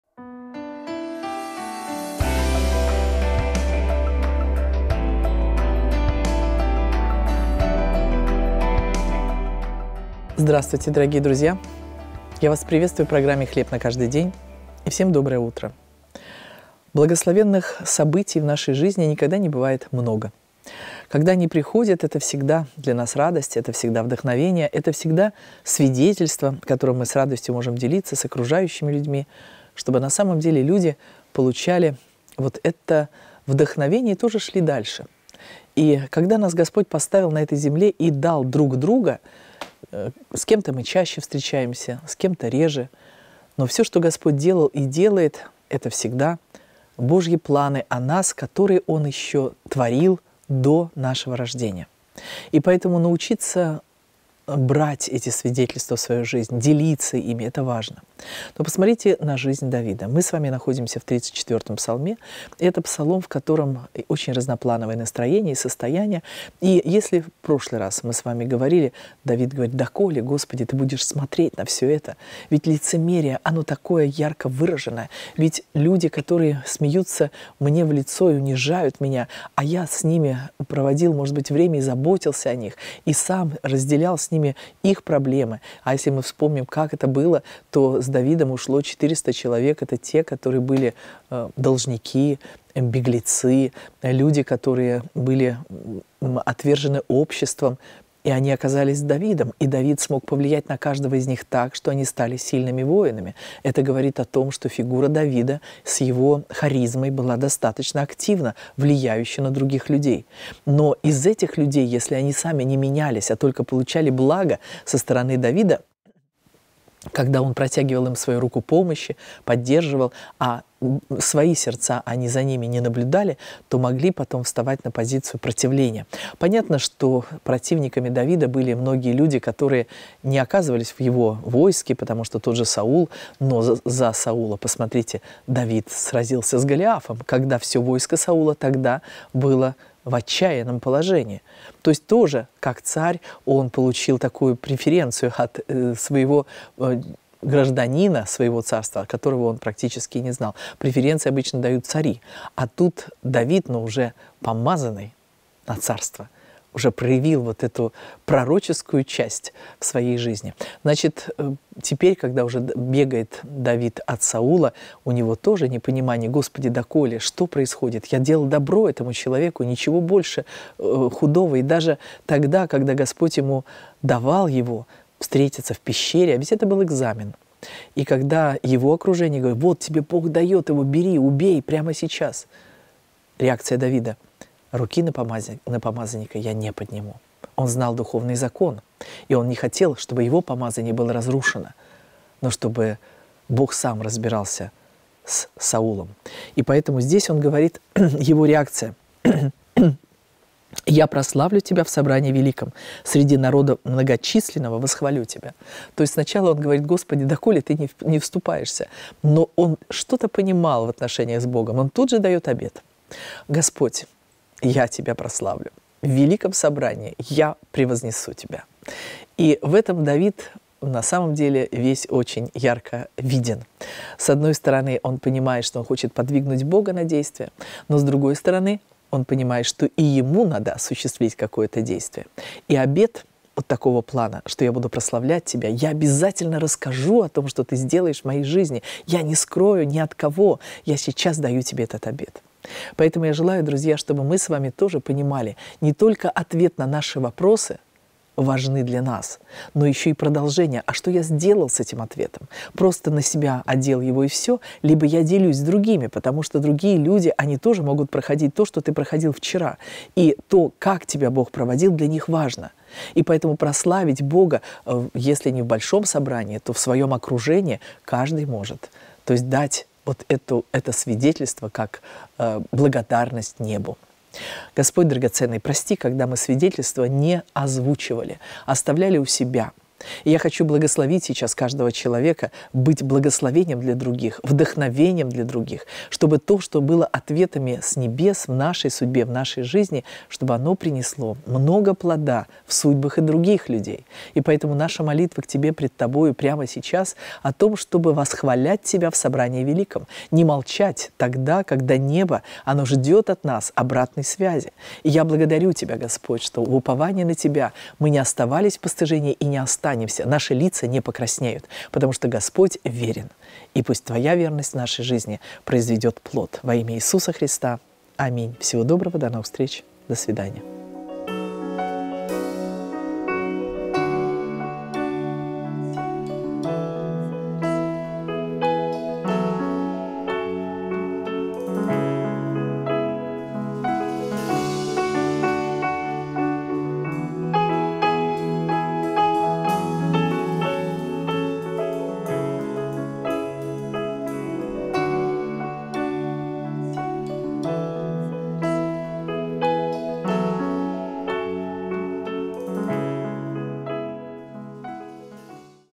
2580. Хлеб на каждый день. Псалом 34:18
2580. Хлеб на каждый день. Псалом 34:18

"18. Я прославлю Тебя в собрании великом, среди народа многочисленного восхвалю Тебя…" (Псалтирь 34:18).